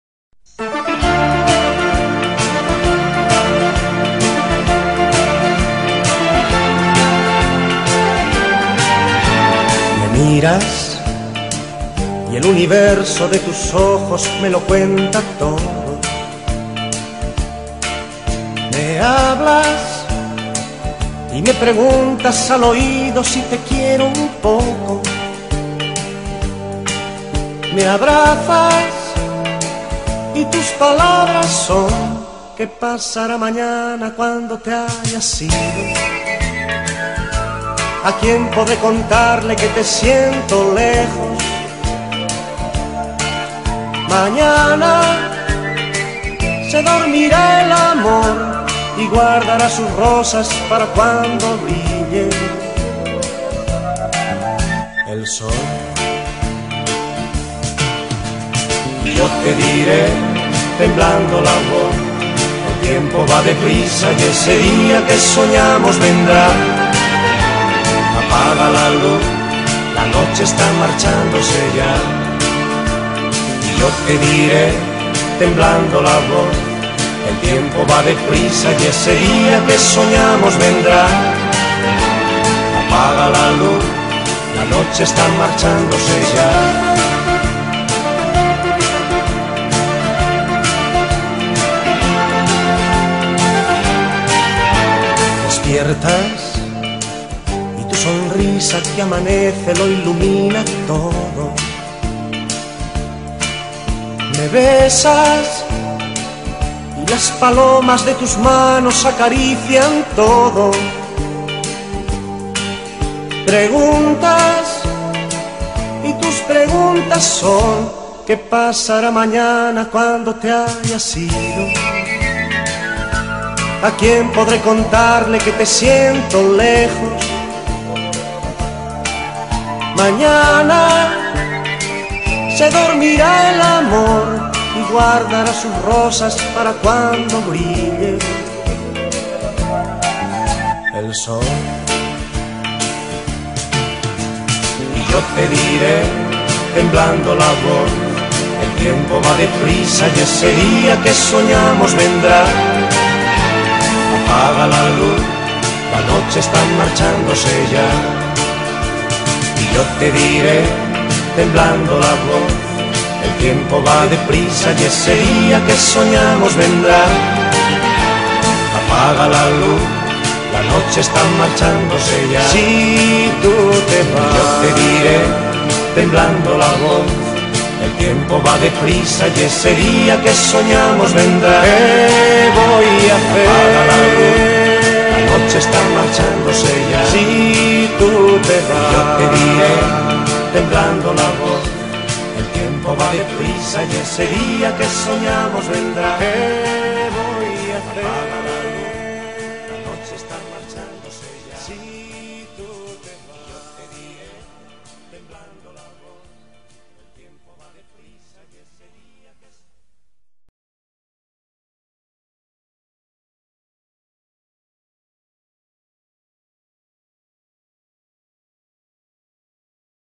FELIZ DIA INTERNACIONAL DE LA MUJER - 8 DE MARZO
EN HOMENAJE A TODAS LAS MUJERES ( MADRE , HERMANAS , TIAS , PRIMAS , AMIGAS  . . . ETC ) DESEANDOLES MUY FELIZ DIA .